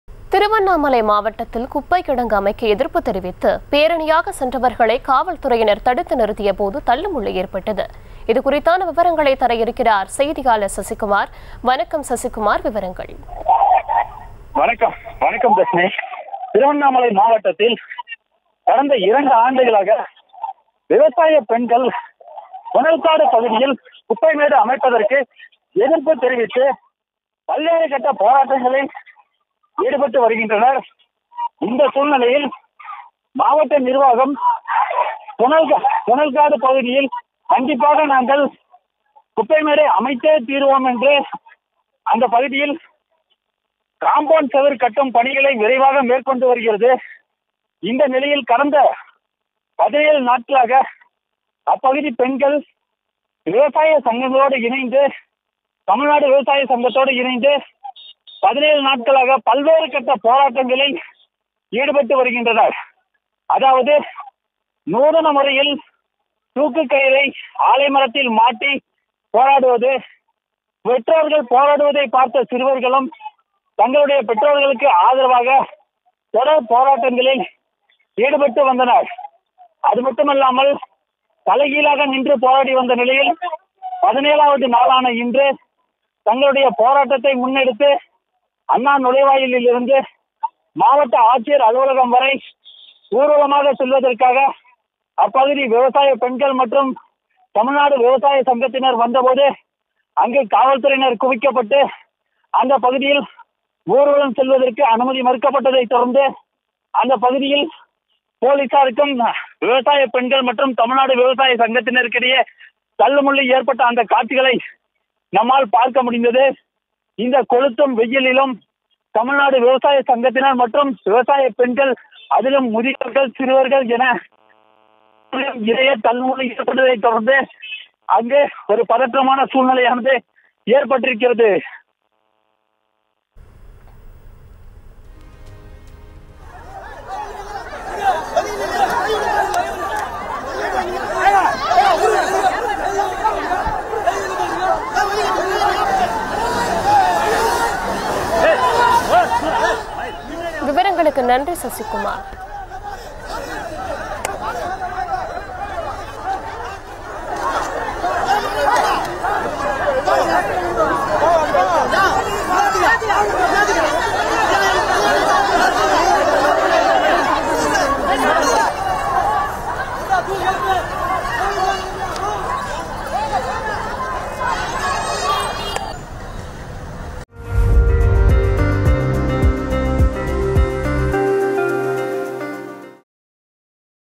Kuppai kidangu அமைக்க எதிர்ப்பு.. பேரணியாக சென்ற போராட்டக்காரர்கள்.. போலீசாரிடையே கடும் தள்ளுமுள்ளு
குப்பை கிடங்கு அமைக்க எதிர்ப்பு.. பேரணியாக சென்ற போராட்டக்காரர்கள்.. போலீசாரிடையே கடும் தள்ளுமுள்ளு